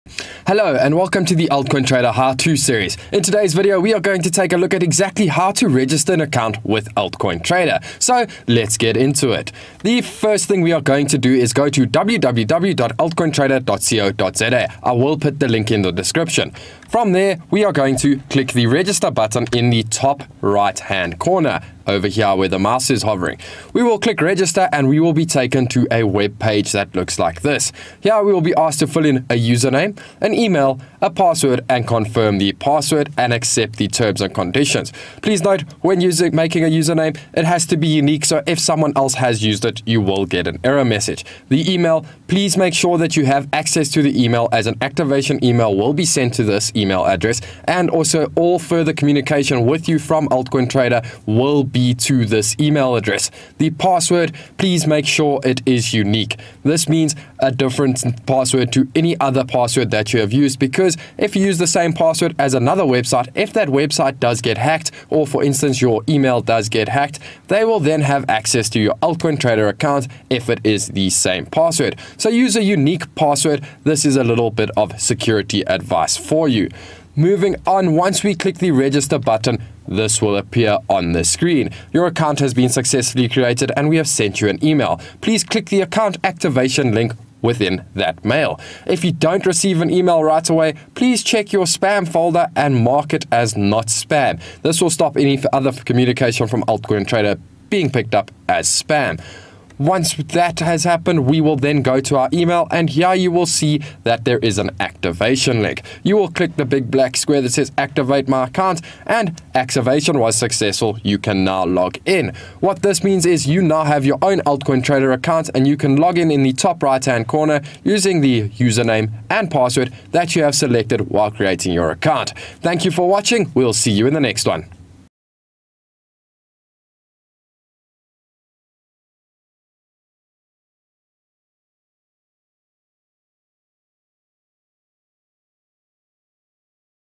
How to register an account with AltCoinTrader!
Welcome to the AltCoinTrader How To Series. In this video we will be looking into how to create your very own AltCoinTrader Account.